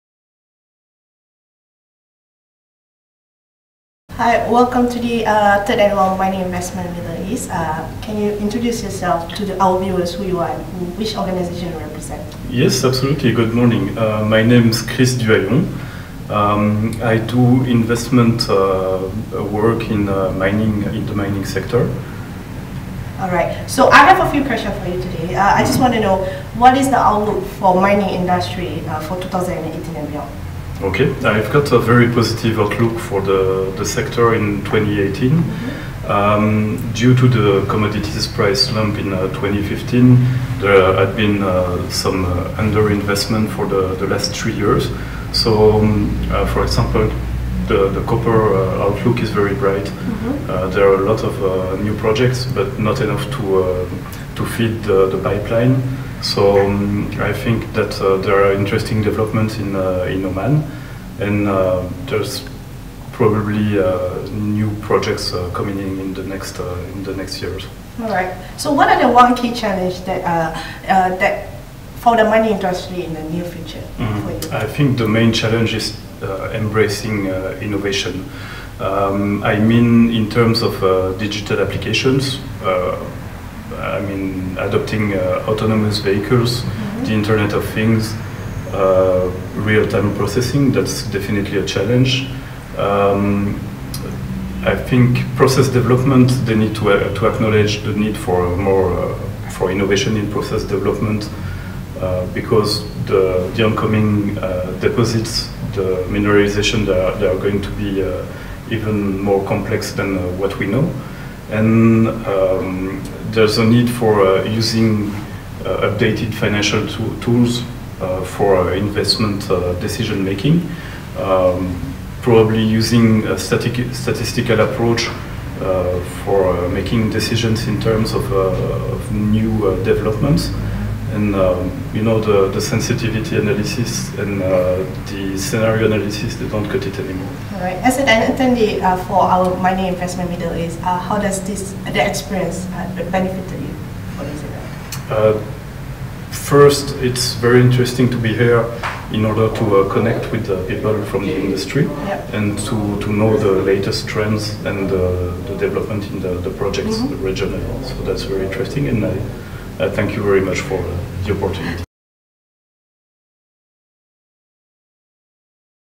Interview with Chris Duhayon from Kanoo Group at Mining Investment Middle East 2018 in Dubai
Interview with Chris Duhayon from Kanoo Group at Mining Investment Middle East Conference 2018 in Dubai